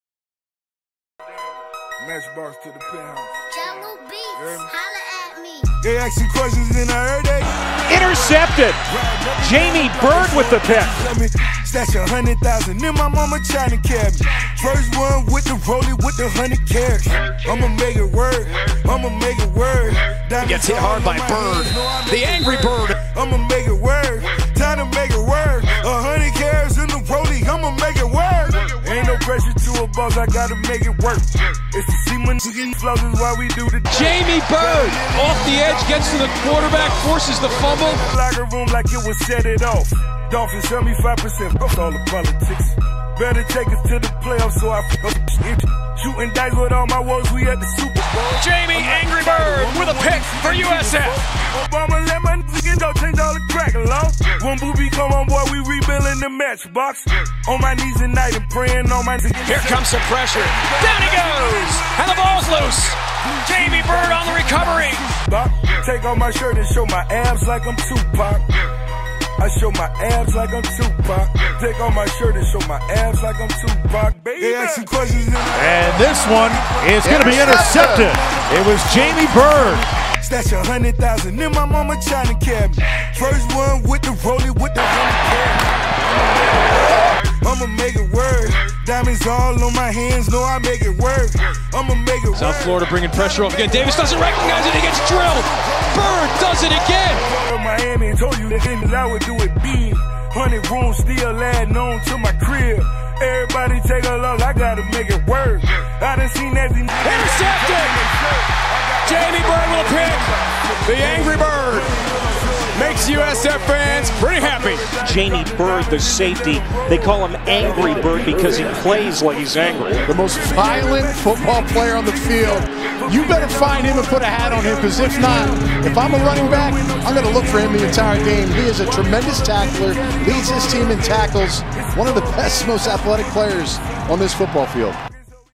Jamie Byrd Official USF Highlight
Official USF highlight of Jamie Byrd. A safety from Dade City, FL. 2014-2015